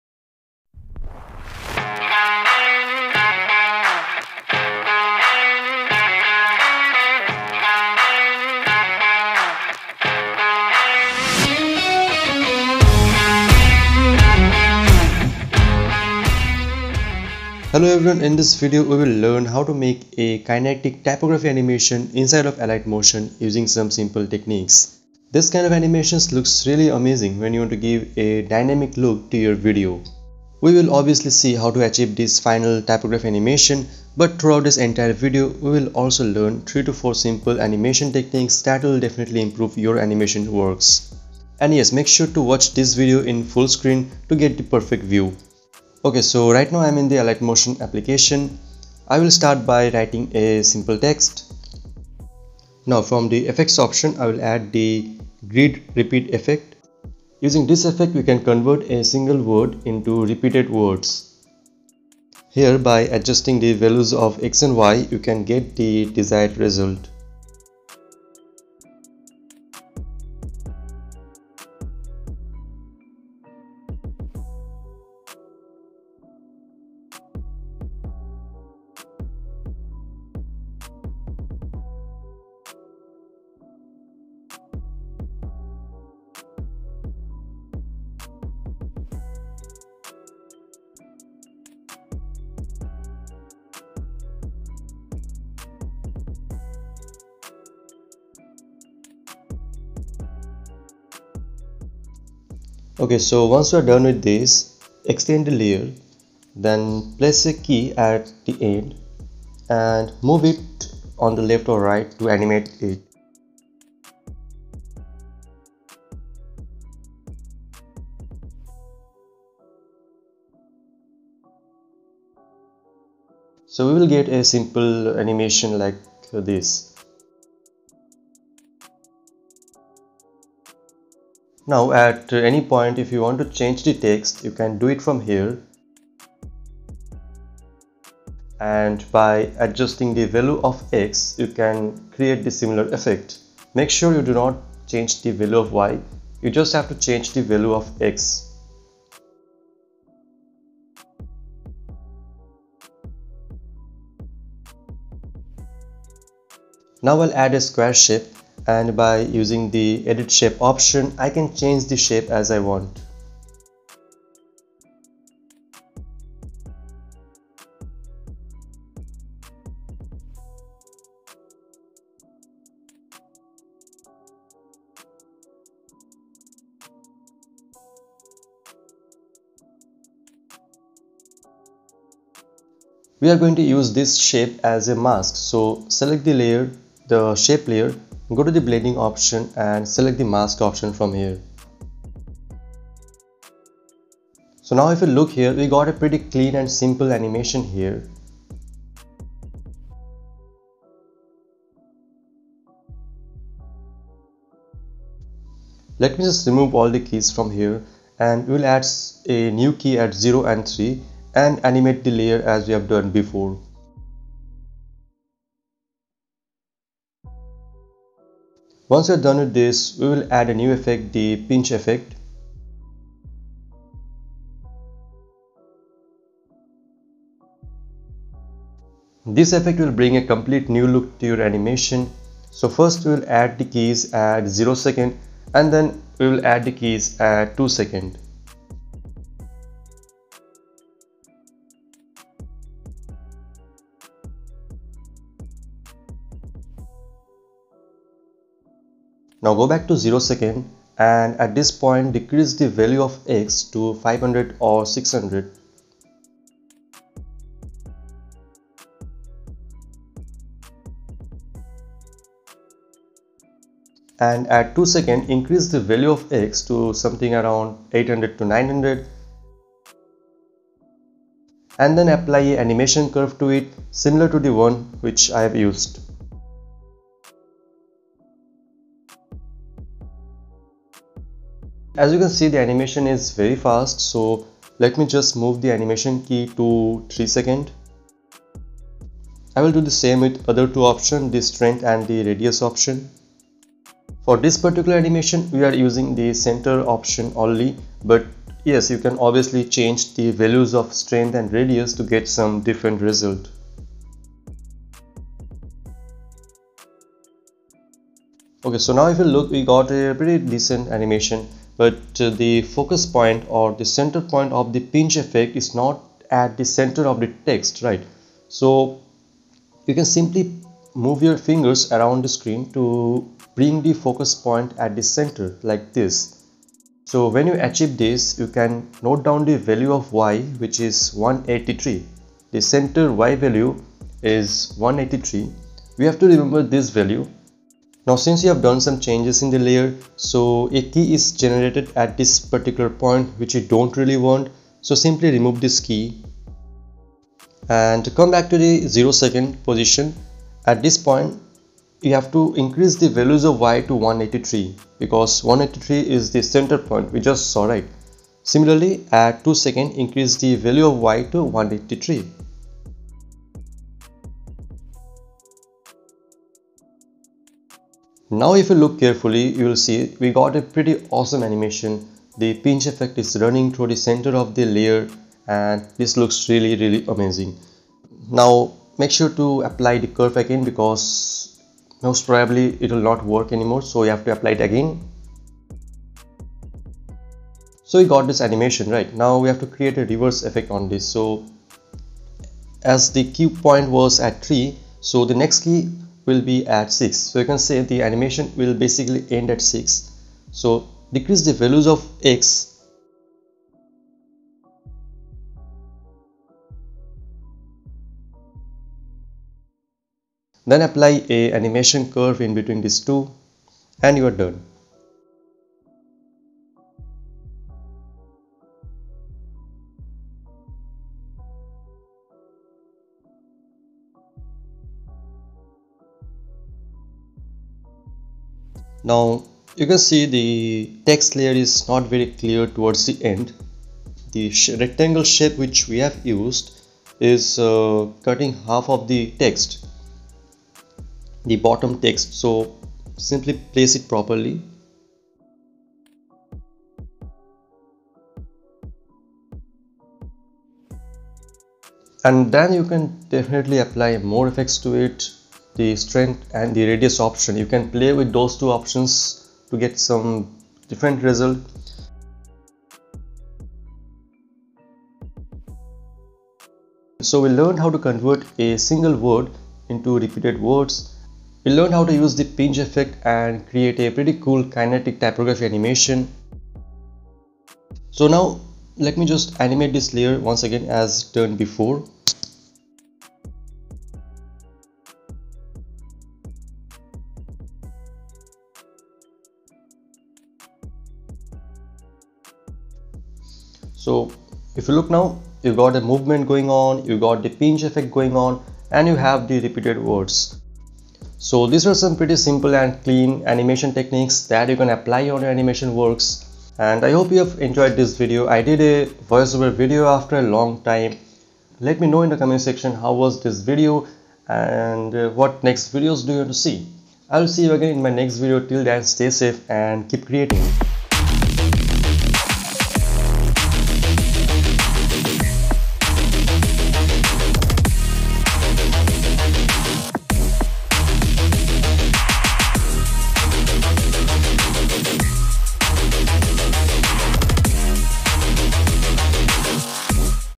Kinetic Typography Animation in Alight Motion | Alight Motion Tutorial
In this video we will learn how to make Kinetic Typography inside of Alight Motion using some simple techniques.

     𝐂𝐡𝐞𝐜𝐤 𝐭𝐡𝐞𝐬𝐞 𝐏𝐥𝐚𝐲𝐥𝐢𝐬𝐭 𝐅𝐨𝐫 𝐌𝐨𝐫𝐞 𝐇𝐞𝐥𝐩𝐟𝐮𝐥
     𝐕𝐢𝐝𝐞𝐨𝐬.

     ➵ 𝐇𝐢𝐫𝐞 𝐌𝐞
          𝐌𝐲 𝐥𝐨𝐠𝐨 𝐰𝐨𝐫𝐤𝐬:

➤ 𝗖𝗢𝗡𝗡𝗘𝗖𝗧

➤ 𝗠𝗨𝗦𝗜𝗖 𝗖𝗥𝗘𝗗𝗜𝗧𝗦